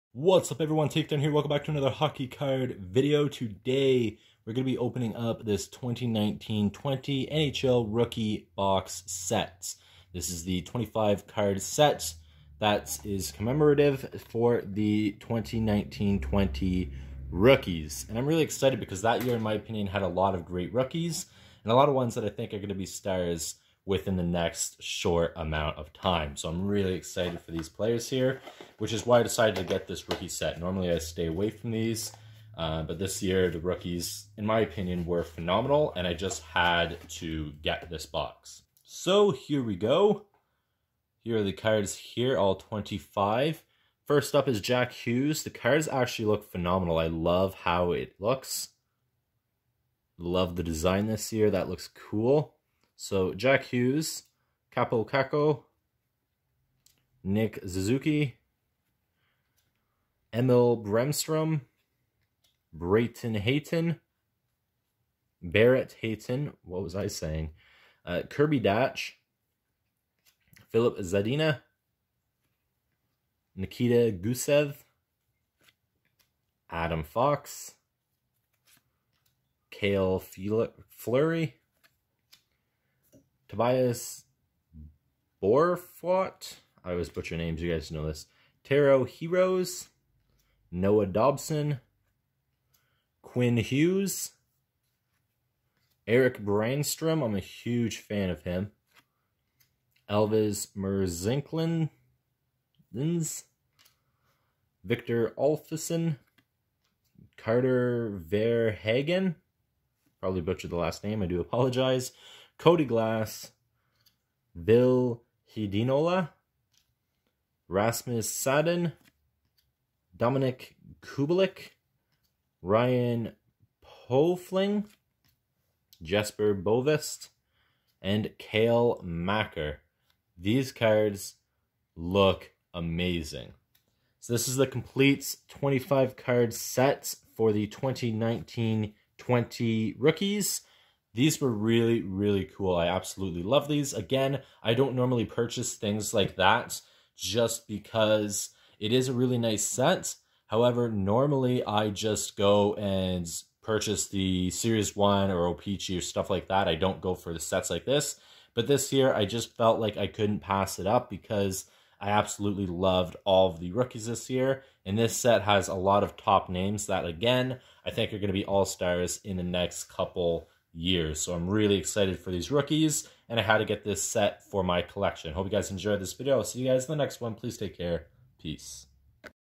2019-20 NHL ROOKIE SET || HOCKEY CARDS
I finally got my hands on the 2019-20 Rookie box set! This was my favorite rookie class in recent years!

#2019-20nhlrookies